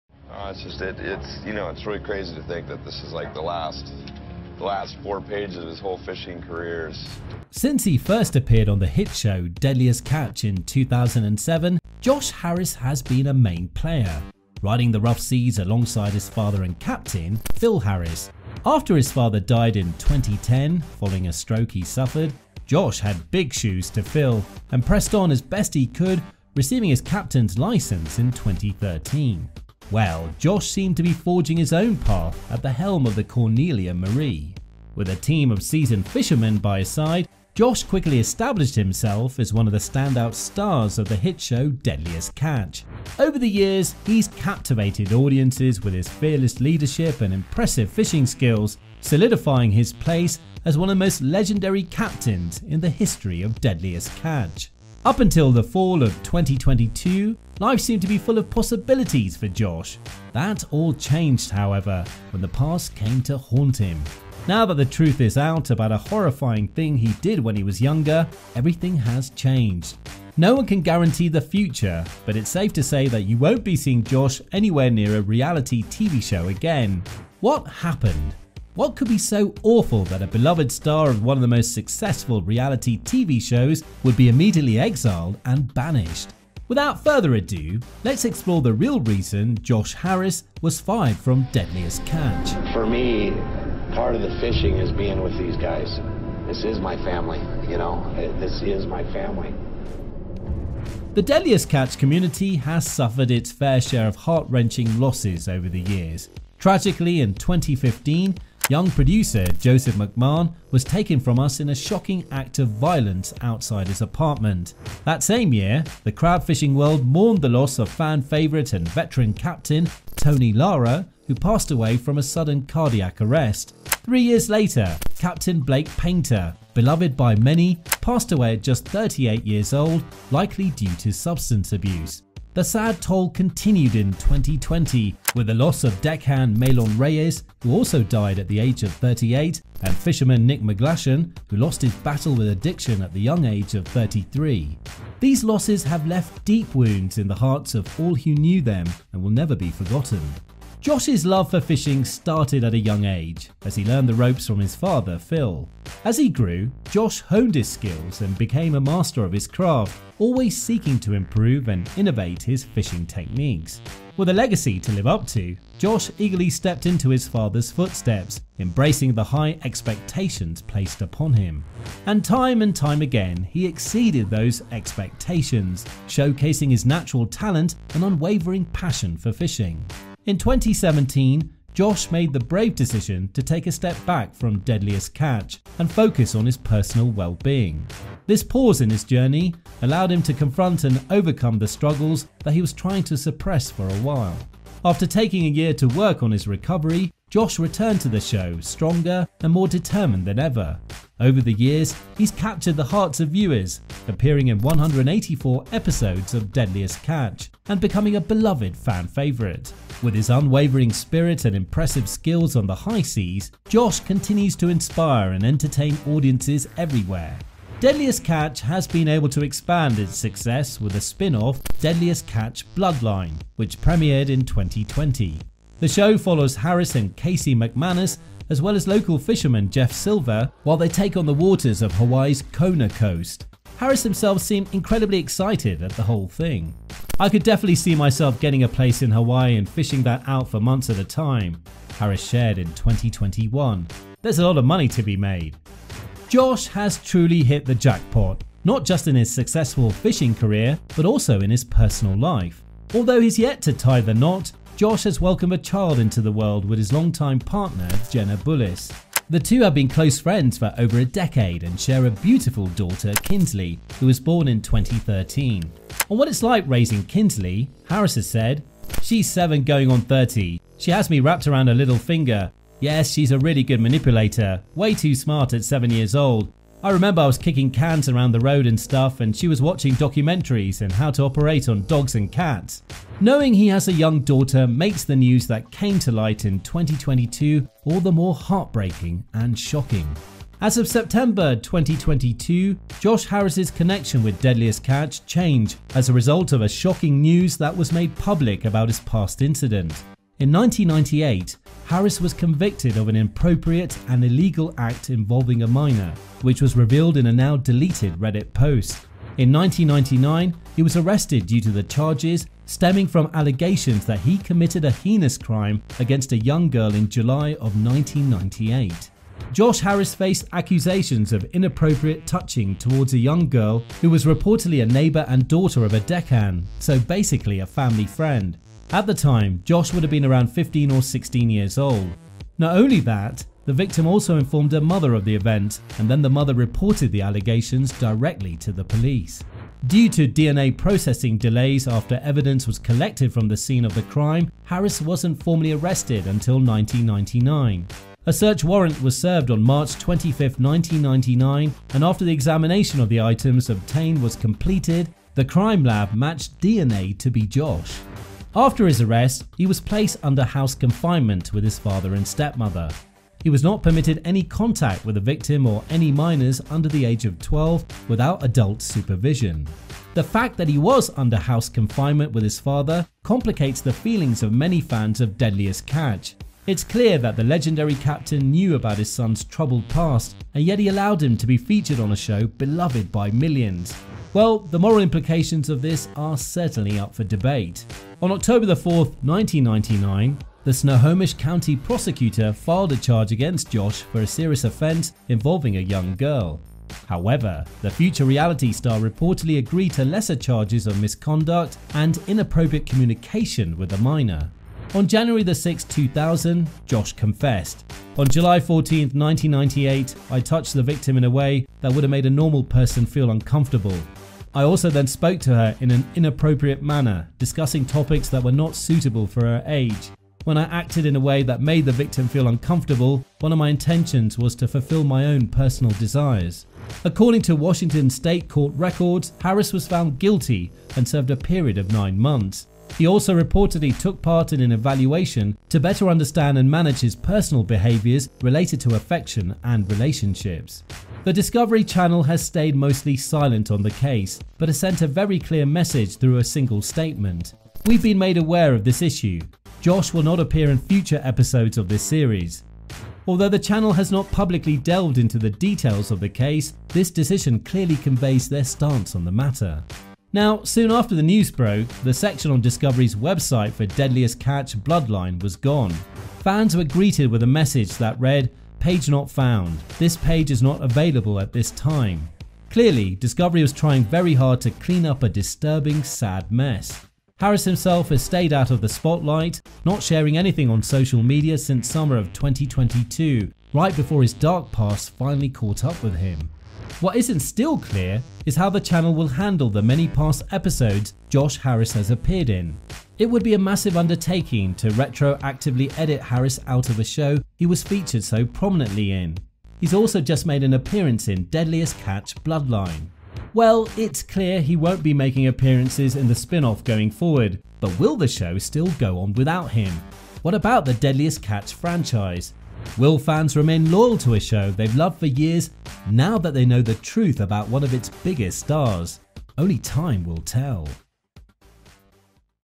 The Real Reason Josh Harris Was Fired From Deadliest Catch
Since he first appeared on the hit show Deadliest Catch in 2007, Josh Harris has been a main player, riding the rough seas alongside his father and captain, Phil Harris. After his father died in 2010 following a stroke he suffered, Josh had big shoes to fill and pressed on as best as he could, receiving his captain’s license in 2013.

Well. Josh seemed to be forging his own path at the helm of the Cornelia Marie. With a team of seasoned fishermen by his side, Josh quickly established himself as one of the standout stars of the hit show "Deadliest Catch." Over the years, he has captivated audiences with his fearless leadership and impressive fishing skills, solidifying his place as one of the most legendary captains in the history of "Deadliest Catch."

Up until the fall of 2022, life seemed to be full of possibilities for Josh. That all changed, however, when the past came to haunt him. Now that the truth is out about a horrifying thing he did when he was younger, everything has changed. No one can guarantee the future, but it’s safe to say that we won’t be seeing Josh anywhere near a reality TV show again. What happened? What could be so awful that a beloved star of one of the most successful reality TV shows would be immediately exiled and banished?

Without further ado, let’s explore the real reason Josh Harris was fired from Deadliest Catch!